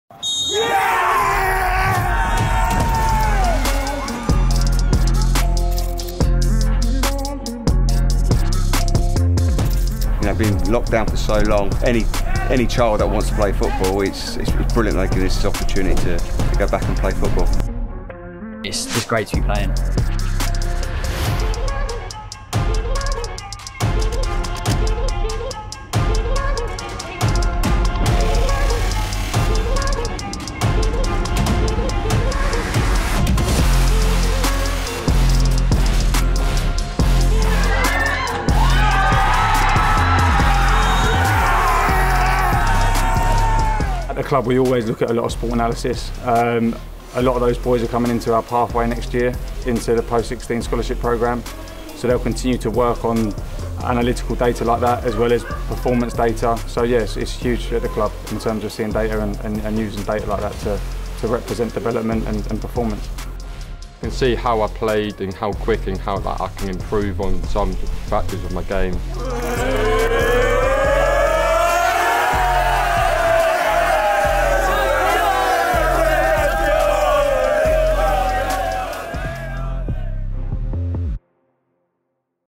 Junior Premier League x STATSports Under-15 FINAL 2019-2020
Bromley FC 2 - 1 Leighton Town Youth Football Club U15s

Players from both sides wore STATSports' Team Series devices during the final, played at Gloucester City AFC's Meadow Park.

For more info on STATSports' partnership with The Junior Premier League,